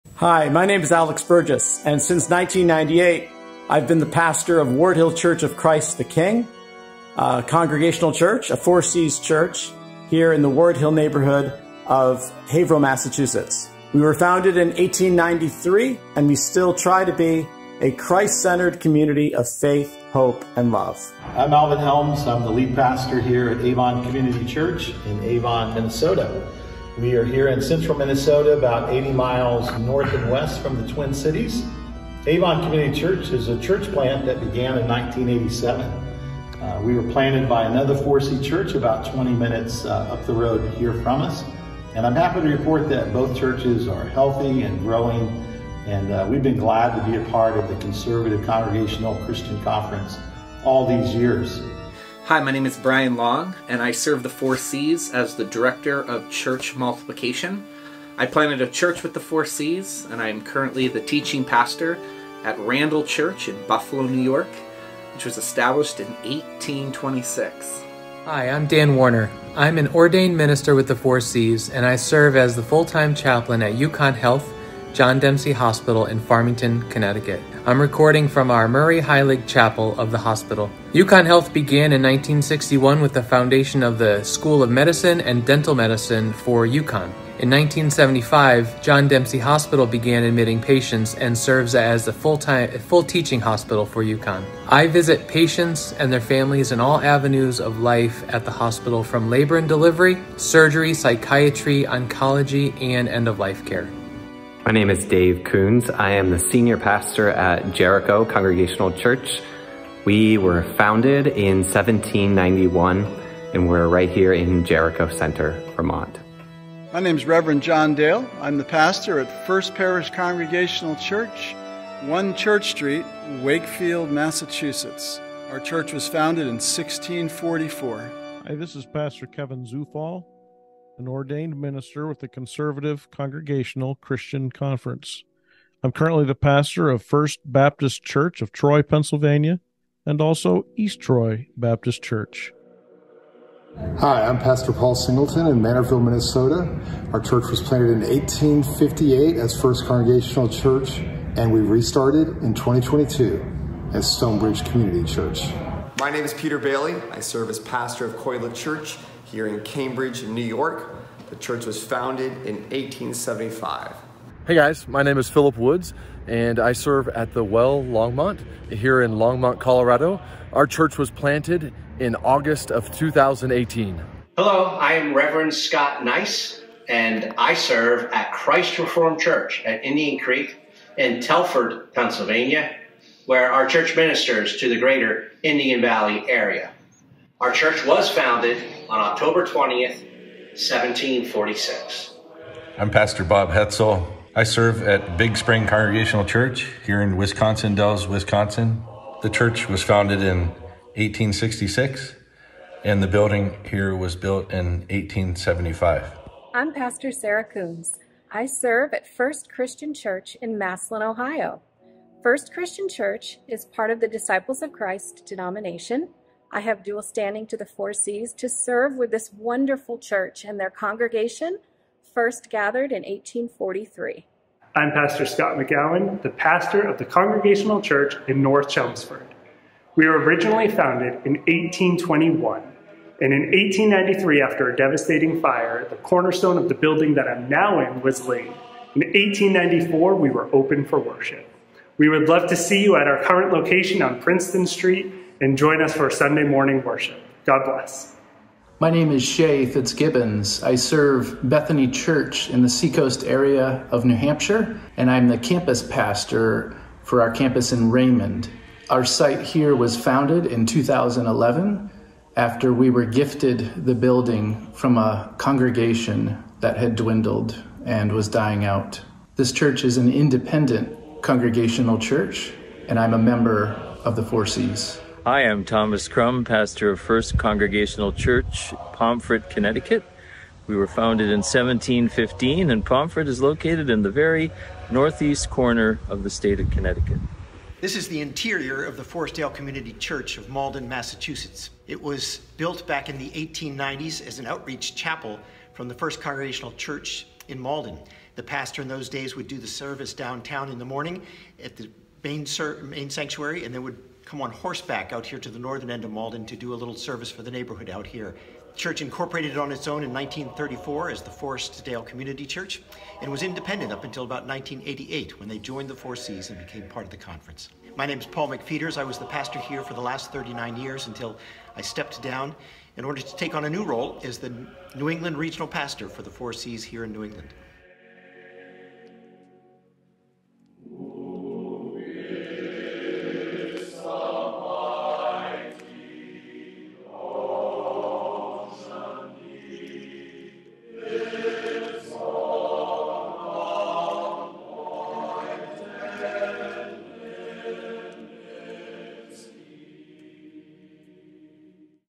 A Pastors and Churches Tour of the Conservative Congregational Christian Conference (CCCC) Part 2
Here's a quick snapshot of some of our CCCC churches, churches where CCCC ministers serve, and even a hospital where a CCCC chaplain works. You can find a complete list of the churches/organizations in the video below.

✅ If you would like to add your church, please watch this video about how to submit